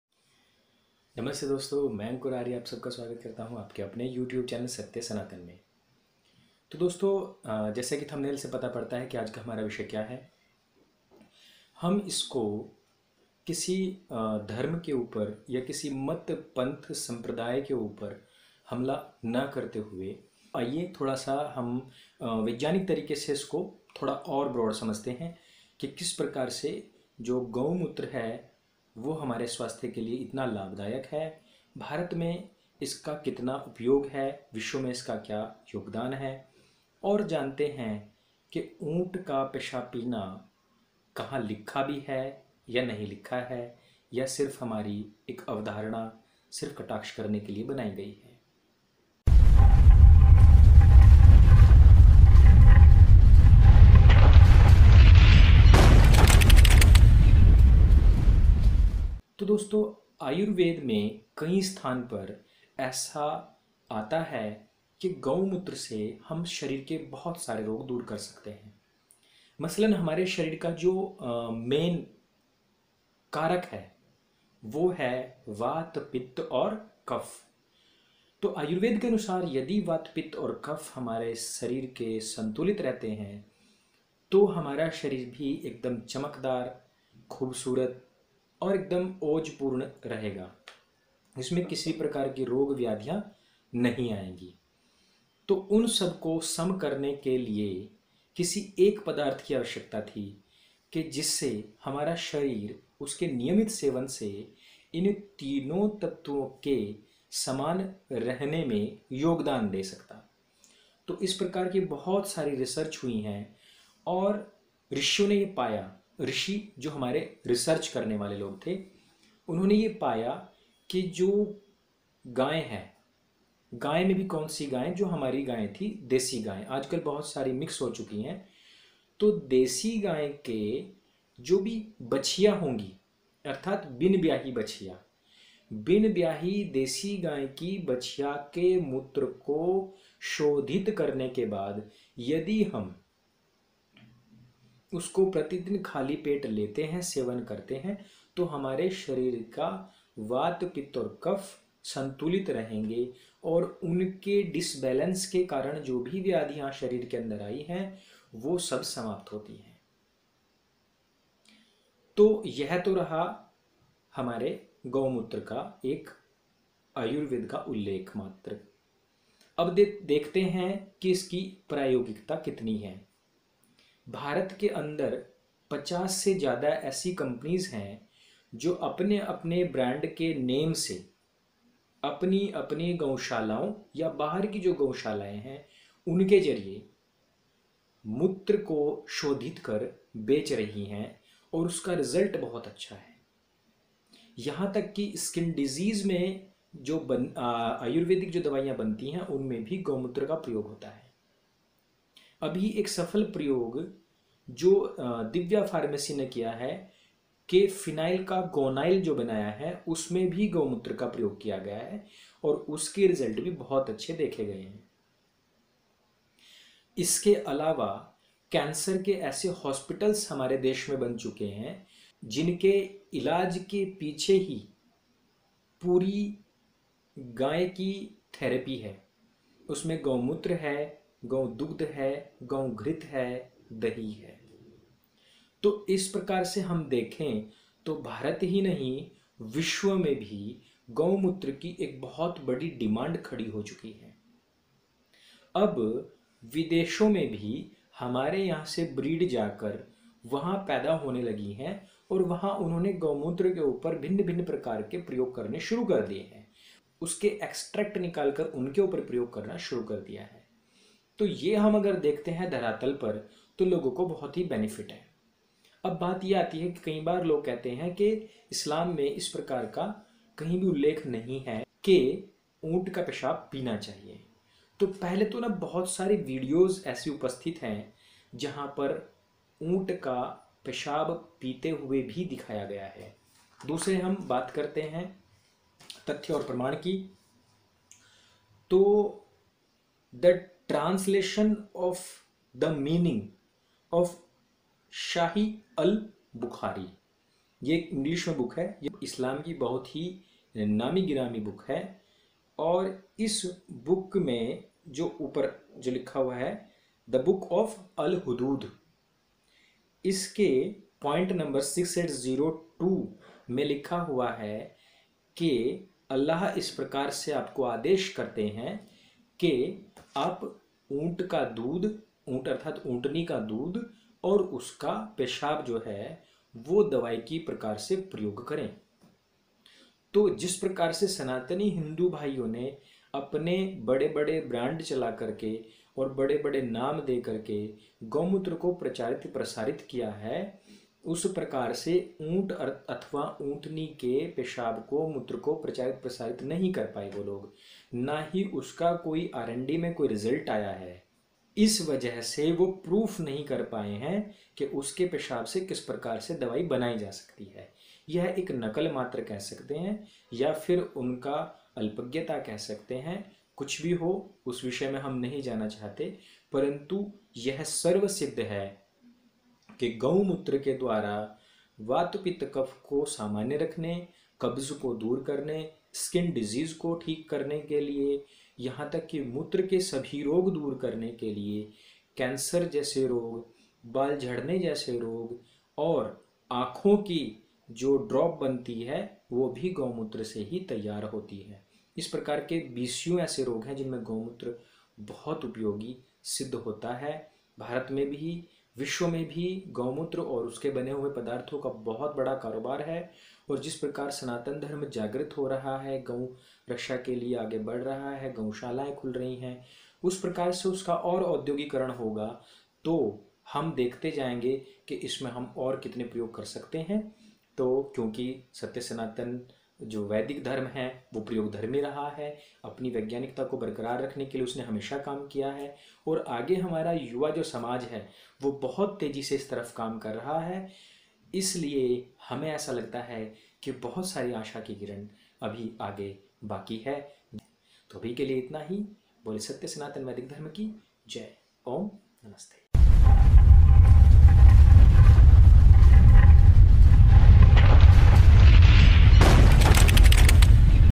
OTT गौमूत्र हराम,ऊंट के पेशाब में पिस्ता बादाम? GAU VIGYAN VS ISLAMIK AGYAAN
सत्य सनातन वैदिक धर्म की जय।

मेरे प्यारे दोस्तो सादर नमस्ते।
जो मुस्लिम लोग हिन्दुओं को गौधन अर्क पीने पर गालियां देते हैं उनके लिए खास है यह वीडियो।